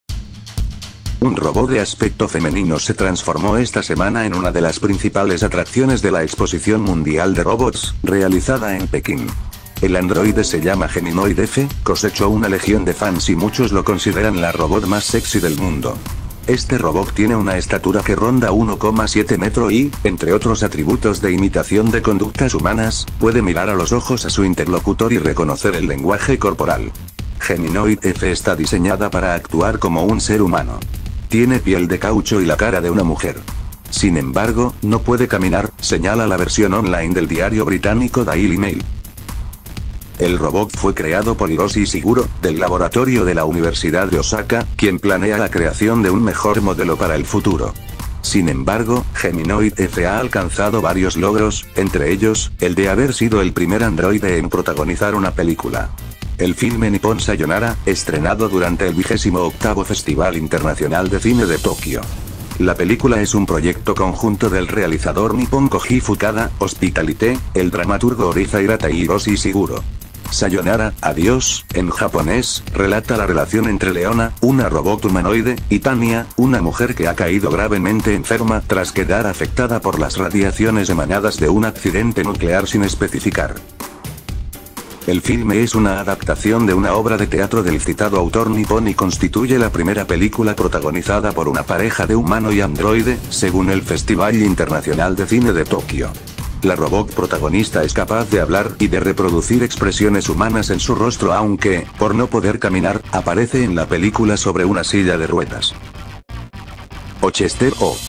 La Robot actriz Que esta causando furor
Se llama  Geminoid F. Mide 1,70 metro,  imita gestos  humanos y reconocer  el lenguaje corporal. Ya prota onizó una película y tiene sus fans.